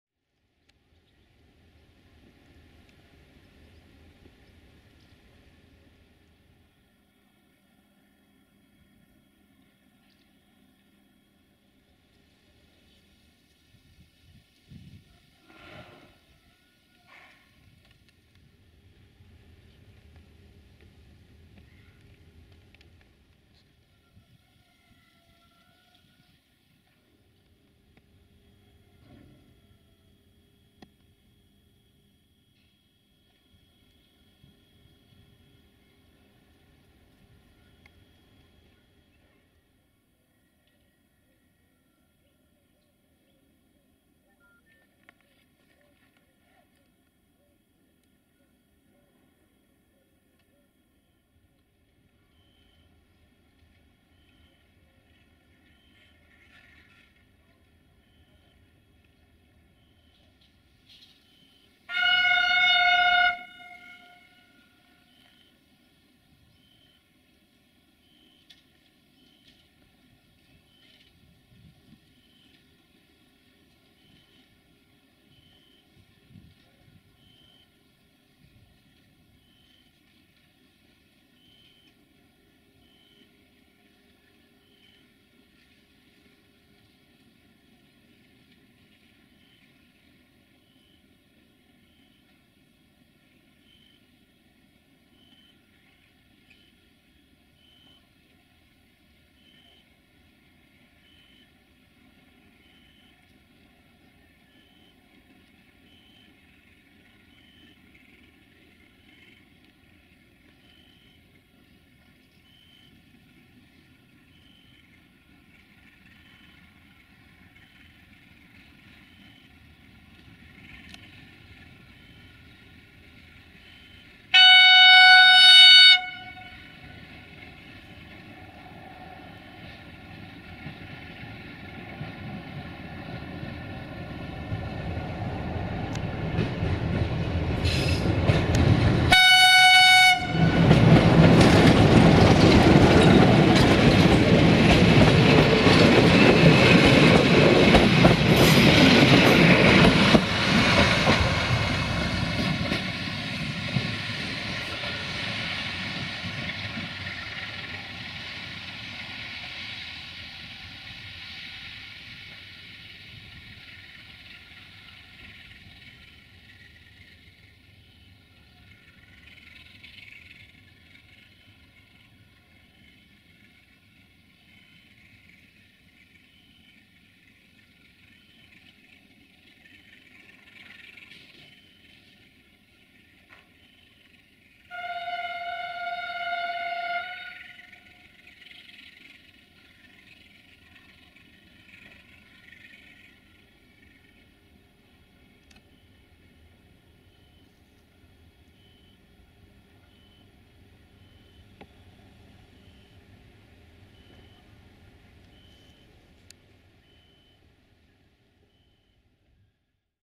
060-DA 60-0809-3 & R4342 Valea lui Mihai-Oradea in Gara Valea lui Mihai Station - 08 September 2020
060-DA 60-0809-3 & R4342 Valea lui Mihai-Oradea in Gara Valea lui Mihai Station - 08 September 2020

1:R4342 - Valea lui Mihai-Șilindru-Săcuieni Bihor-Diosig-Biharia-Episcopia Bihor-Oradea - 060-DA/LDE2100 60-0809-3 RO-SNTFC

08 September 2020